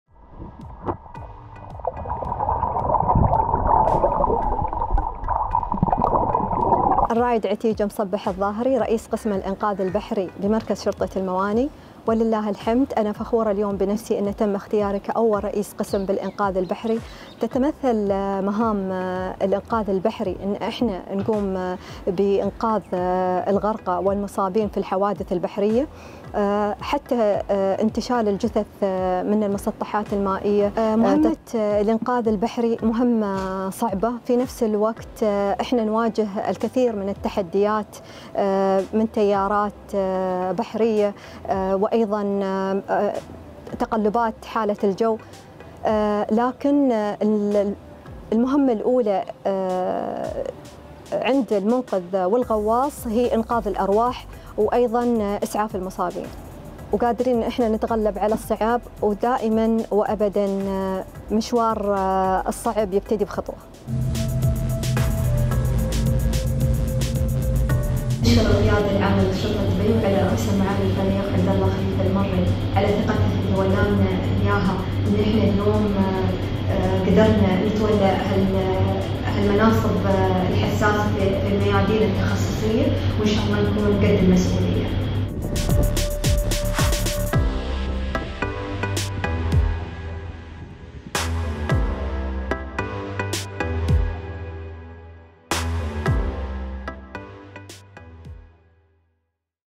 Meet the first Emirati female police officer making waves in the maritime rescue team
Major Ateeqa Musabah Al Dhaheri, a Dubai Police officer, is today the face of the Maritime Rescue Department. She’s the first female member of the team.